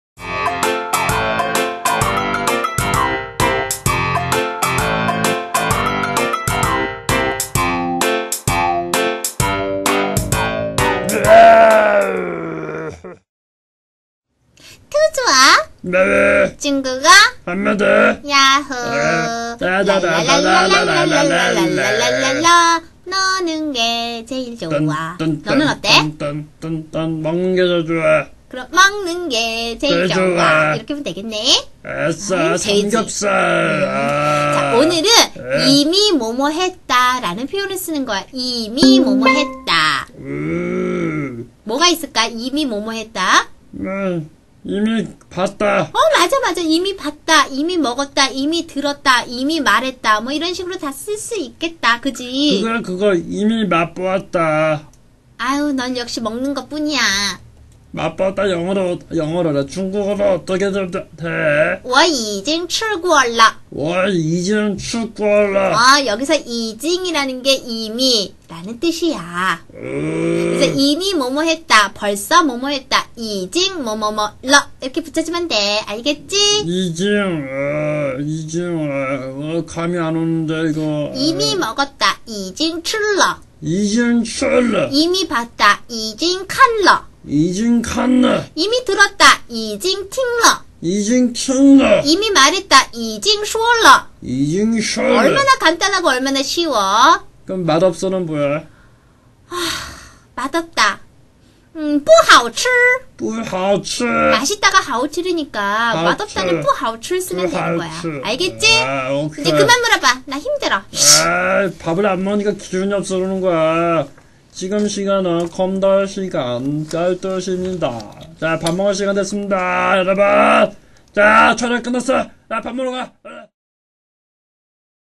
중국어회화 중급 제23회 已经 이미
니하오마~
뚜즈와 마오의 중국어 한 마디입니다

먼저 띄엄띄엄 올려서 죄송합니다..;;

곰돌군(마오)가 깊은 산에 갔다가
날 풀려서 이제 내려왔네요

오늘 배울 표현

已经 이미

그럼, 짜이찌엔~^^

영상이 쪼금이라도 도움되셨다면
"좋아요" 부탁드려요

여러분의 작은 관심이
곰돌군, 토끼샘에게 힘이 됩니다.
                                                -바보뵹곰돌,토끼샘 올림 -